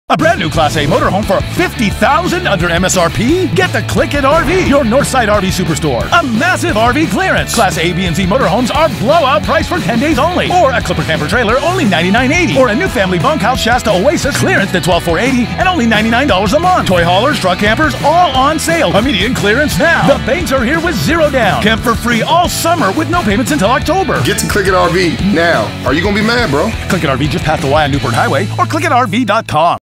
CLICKIT RV 10-DAY CLEARANCE SALE
Clickit RV in North Spokane is having a MASSIVE Inventory Clearance Sale! Motorhomes, travel trailers, toy haulers and fifth-wheels being sold for thousands of dollars off MSRP and No Payments until October! 10 Days Only so come by or call 509/468-5001 today!